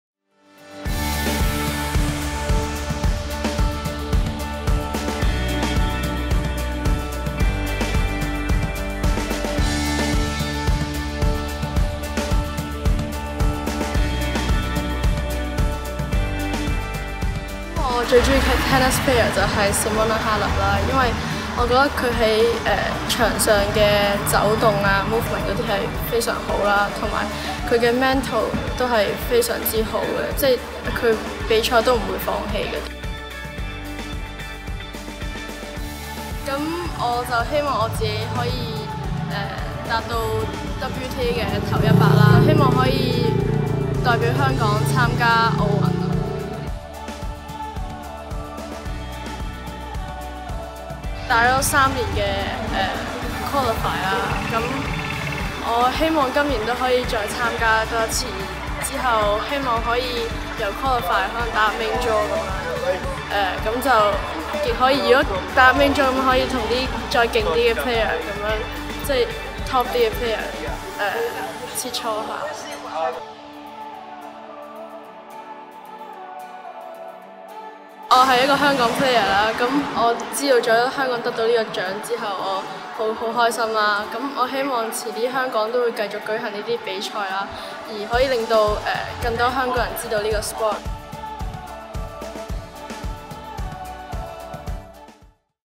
Prudential Hong Kong Tennis Open 2019 - Players' Interview: Cody Wong
Upcoming local talent Cody Wong has been a part of Prudential Hong Kong Tennis Open the last three years through the participation in the Qualifying draw. The teenager has been phenomenal this season, which included a foray into the third round of the Girls Singles at Wimbledon in July. We caught up with Cody in London...

香港網球新星之一的王康怡在過去3年均參加了「保誠香港網球公開賽2019」資格賽。她在這個賽季的表現非常出色，更於上月在溫布頓參加網球錦標賽青少年賽事，打入女子單打賽第三輪。
她跟我們分享了這趟旅程的感受及細說「保誠香港網球公開賽2019」對她的運動生涯有多重要。